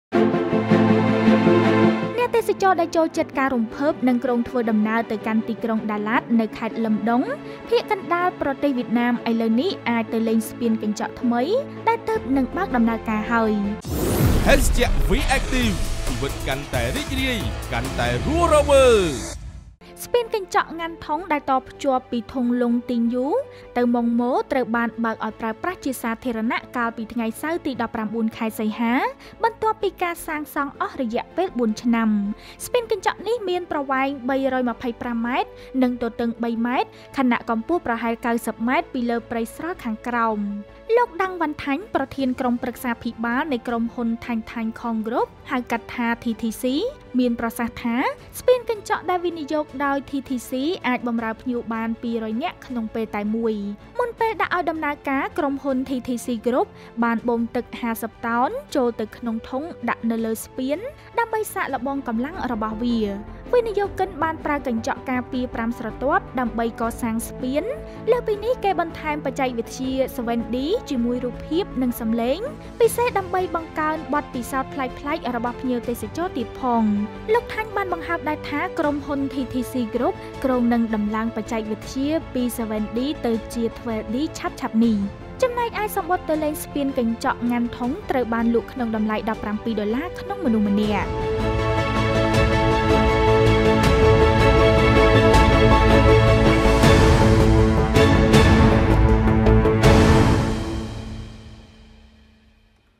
ស្ពានកញ្ចក់ដំបូងនៅទីក្រុងដាឡាត បើកឲ្យទស្សនាជាសាធារណៈហើយ
ស្ពាន​​កញ្ចក់នេះ មាន​ប្រវែង​ ៣២៥ ម៉ែត្រ និង​ទទឹង ៣ ម៉ែត្រ ខណៈ​កម្ពស់​ប្រហែល ៩០ ម៉ែត្រ​ពីលើ​ព្រៃ​ស្រល់​ខាងក្រោម ។